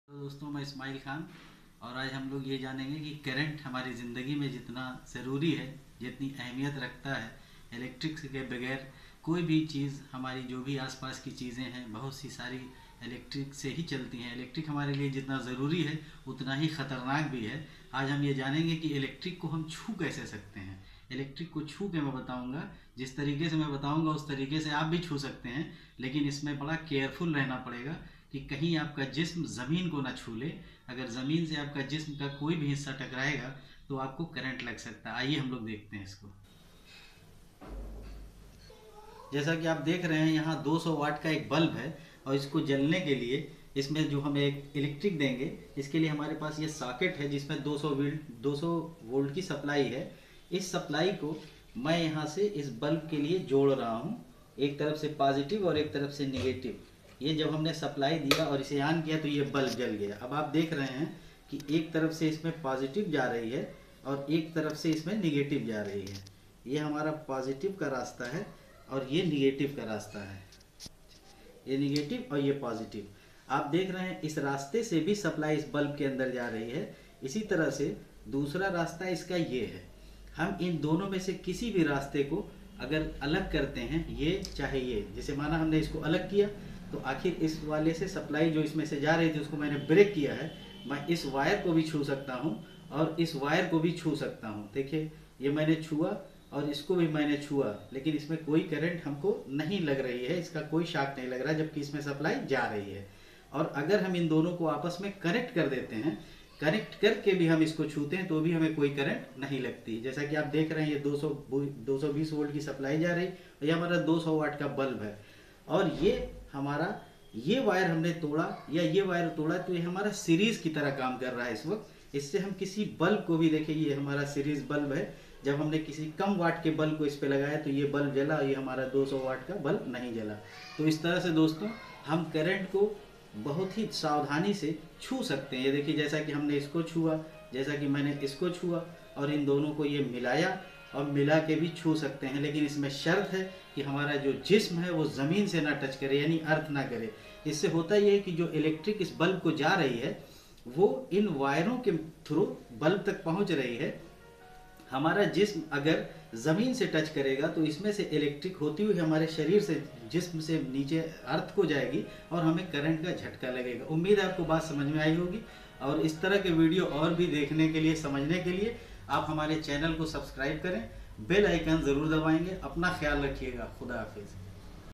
Touch The Electric Current
Hi Friends ,
I M Ismail Khan And All Of You Are Welcome In My YouTube Channel Ideal For Technical
Dosto Maine Is Video me Electric current ko carefully touch karne Ka tareeka samjhaya hai, Refrigeration Air conditioning ya Electrical Field me kaam karne walay New logon ke like ye jaankari bahut zaroori hai,  RAC, Electrical ki knowledge ke liye channel subscribe kijiye

Queries Solved
1 Series Lamp
2 How To Touch Electric
3 Carefully Work Of Electric